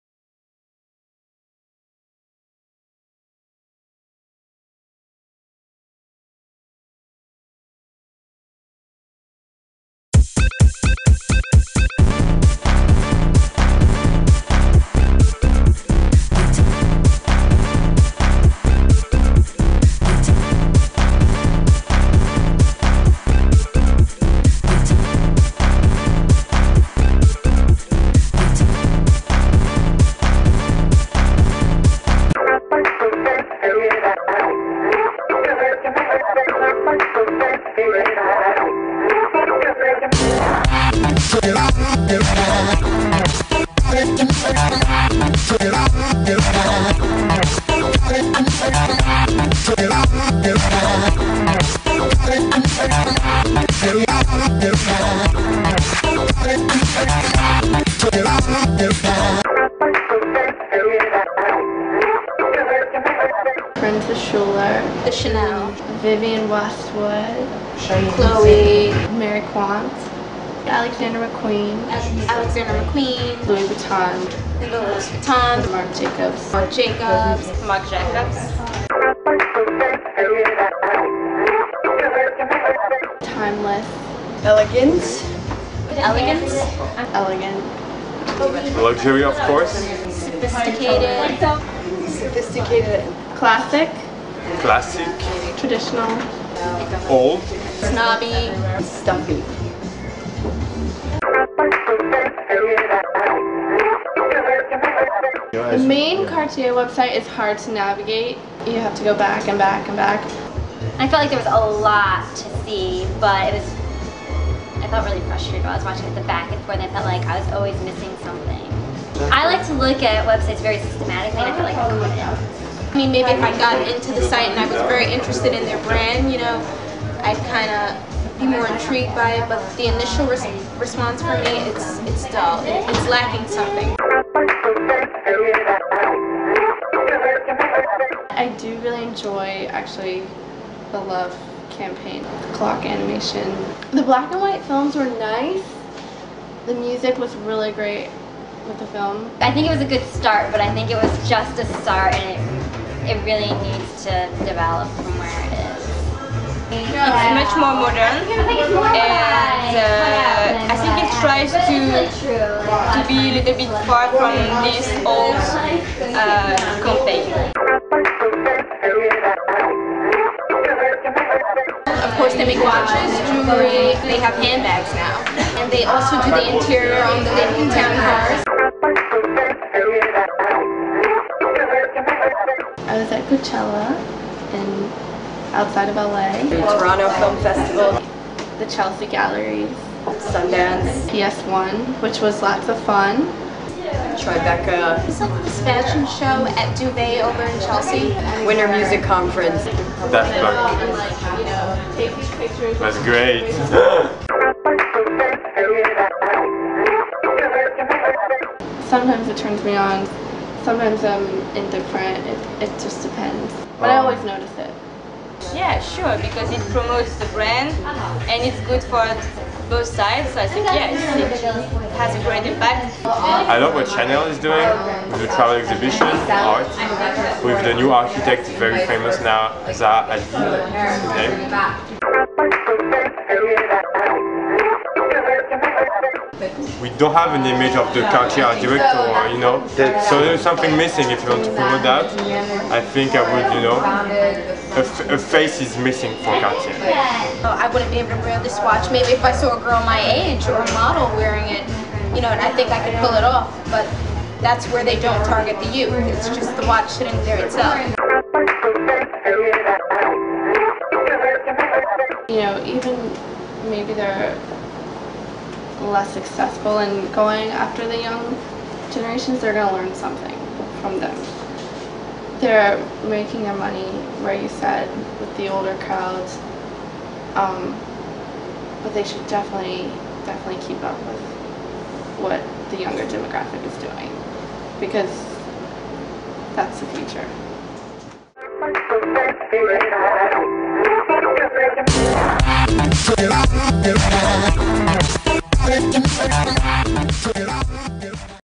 Luxury Goods Focus Group
How to sell to the affluent youth market.